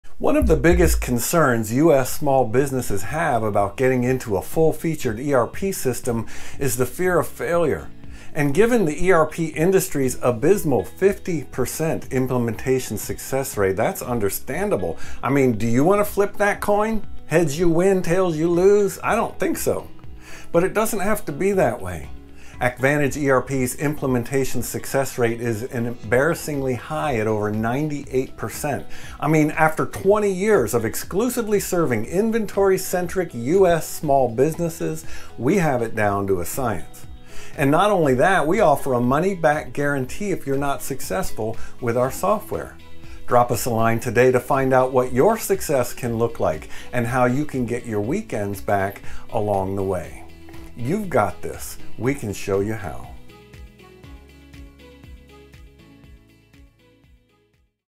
The ERP With the 98% Success Rate - AcctVantage ERP
For small businesses, the idea of implementing a full-featured ERP system can be daunting—especially with the industry’s average success rate of just 50%. Why take a gamble with your business's future?

At AcctVantage ERP, we turn the odds in your favor with an outstanding 98% success rate over the last 20 years, specializing exclusively in inventory-centric small businesses like yours. We’ve perfected the implementation process to a science, and we’re so confident in our approach that we offer a money-back guarantee if you’re not successful.

You've got this, and we're here to guide you every step of the way.